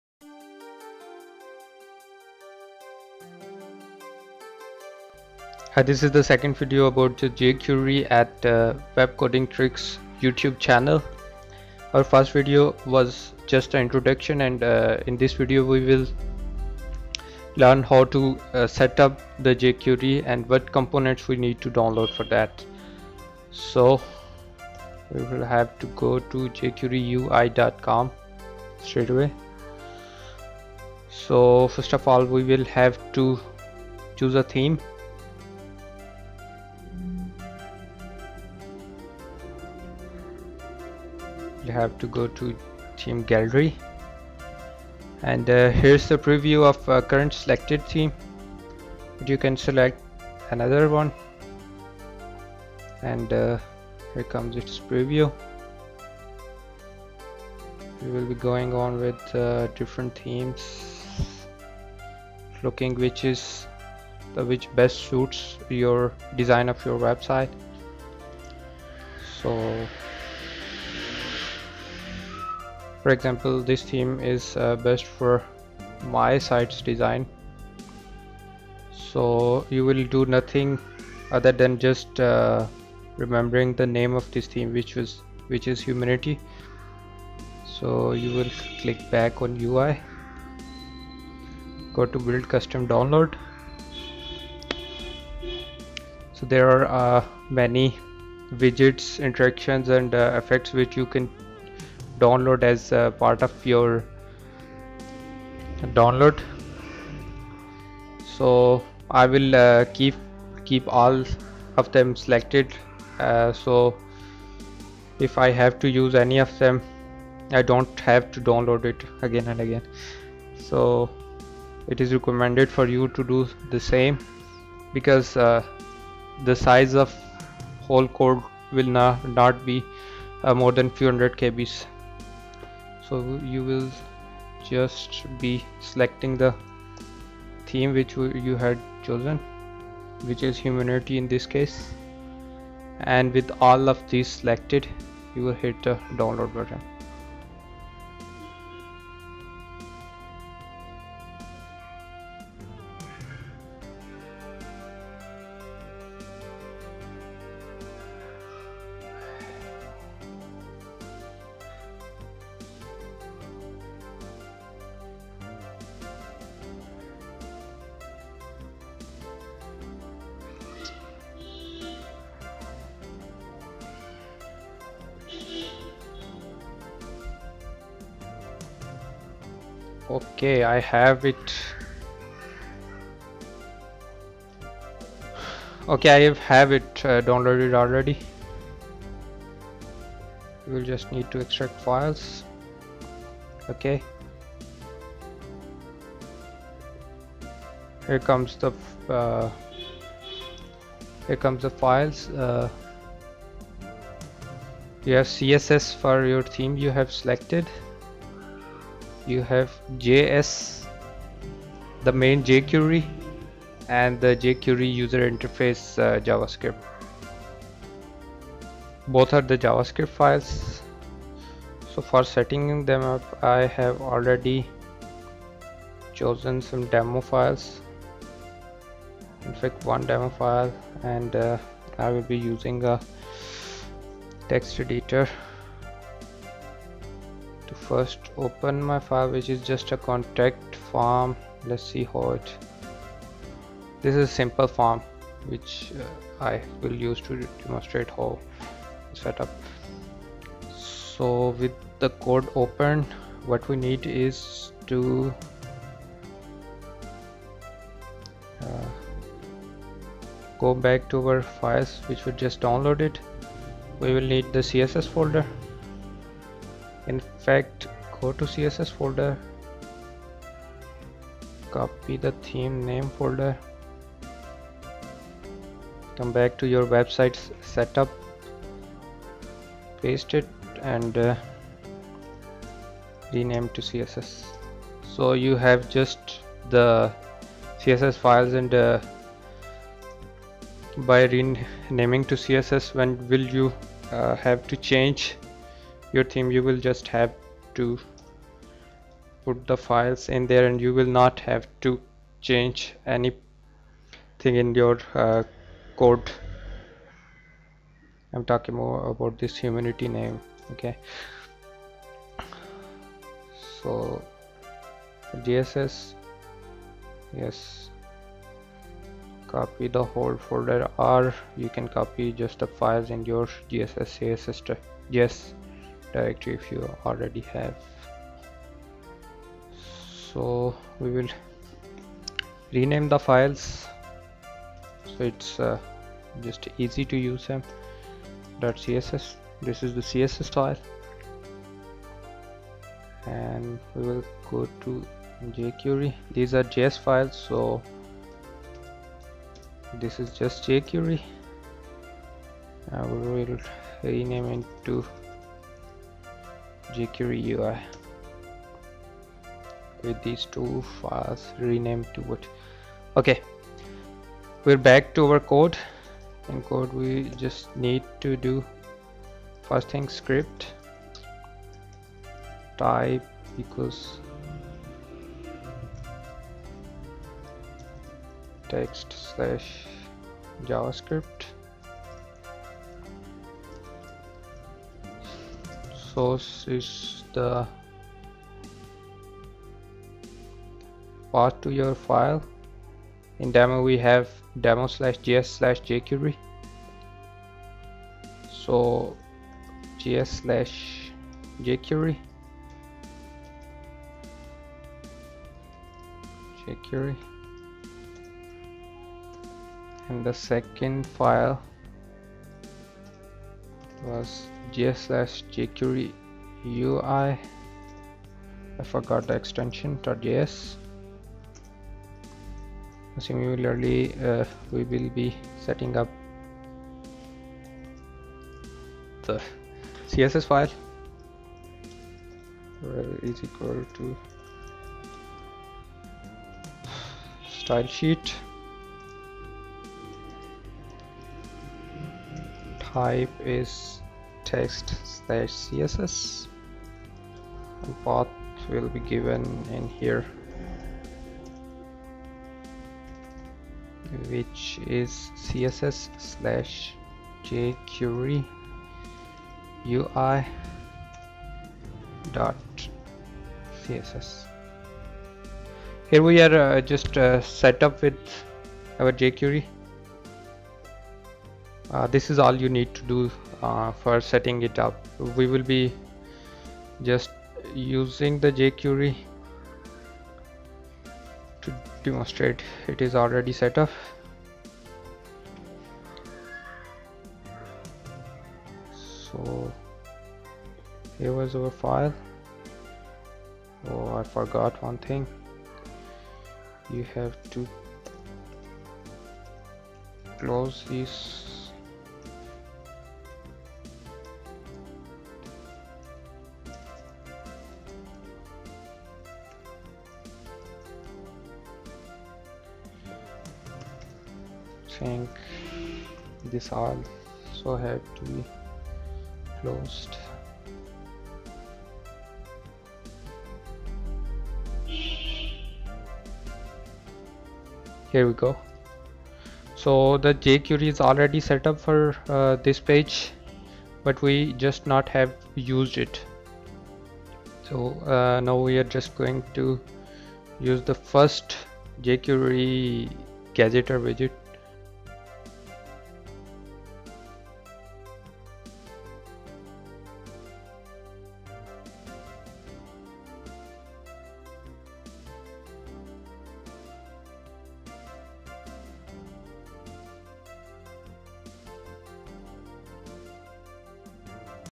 Setup jQuery and JQuery-ui along with theme - jQuery Part 2A (Old)
Complete jQuery, jQuery-ui setup along with theme, steps including choosing required components for download, choose theme as well and finally setup the jquery, along with first use of jQuery.